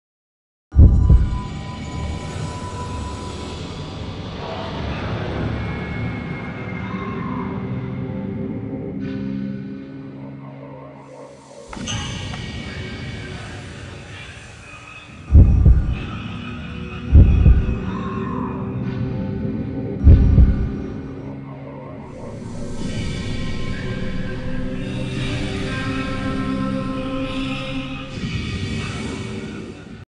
Calamity movie trailer
A university assignment I worked on a while back. Inspiration from Project Zero/Fatal Frame.